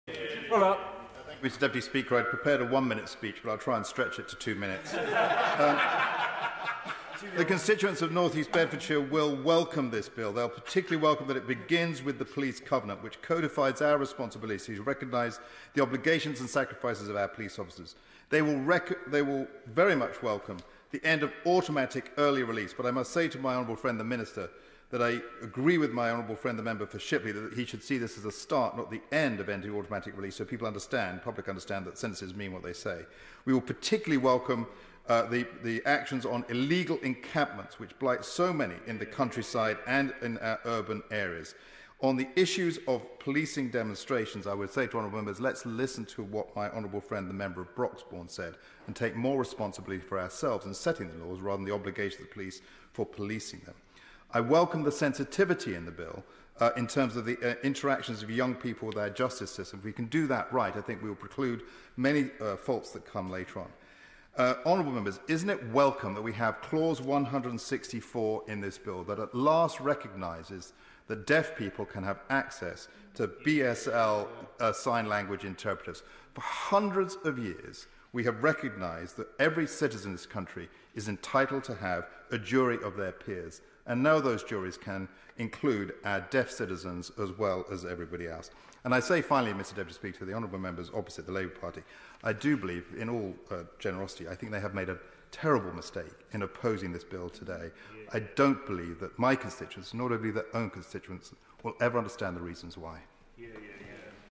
15th March 2021 - Debate on The Police, Crime, Sentencing & Courts Bill
Richard welcomed the provisions laid out in the Police, Crime, Sentencing and Courts Bill and remarked on the terrible mistake The Labour Party had made by opposing the legislation.